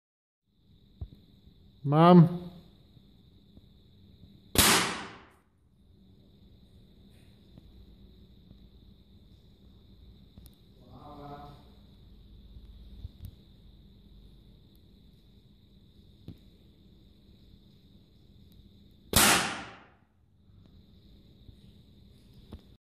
Hit sensor
hit in the thin metal plate triggers a flash which is seen by observer. No neet to carry heavy hardox but just a think 5€ worth of sheet metal.